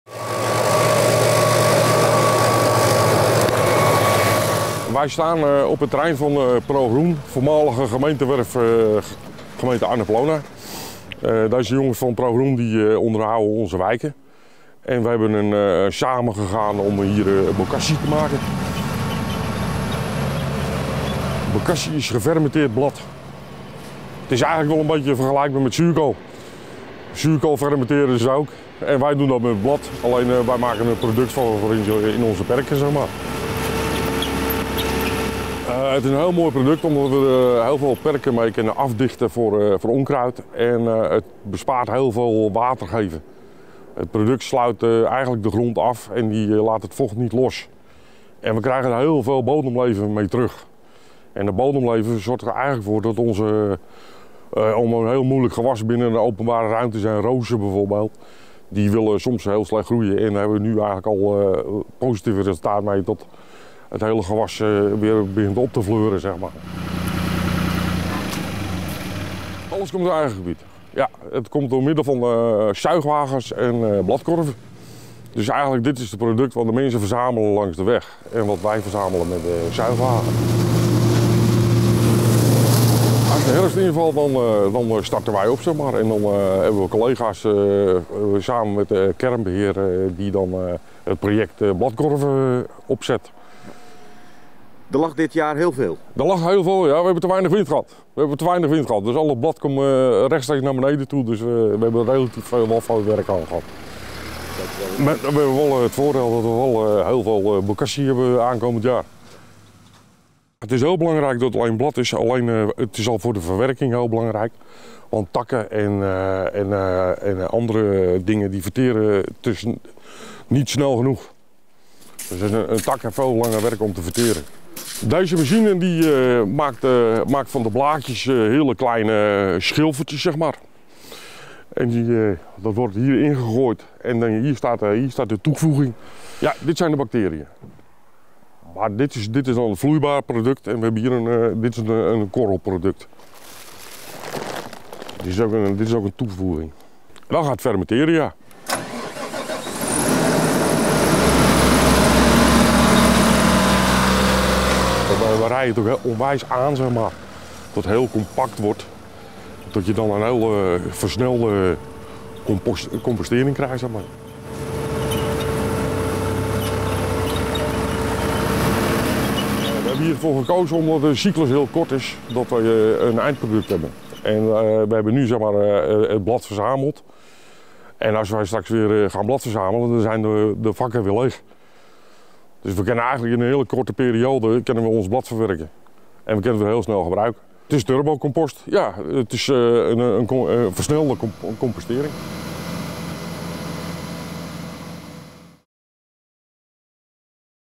Gevallen bladeren zijn de basis voor de supersnelle compost 'bokashi'
Vorig jaar is de gemeentehollandskroon begonnen met het zelf maken van bokaski, een supersnelle compost van gefermenteerde bladeren. Het dekt de grond af en zorgt dat onkruid er niet door kan, het houdt water vast en het zorgt voor meer leven in de bodem.

In de herfst worden de bladeren opgezogen met een speciale machine of verzameld in de bladkorven die op veel plekken in de gemeente te vinden zijn. Dat materiaal wordt vermalen, er worden speciale bacteriën bij gedaan. Daarna gaat er een trekker overheen om het een compacte bult van te maken. In het voorjaar kan de bokashi al worden gebruikt.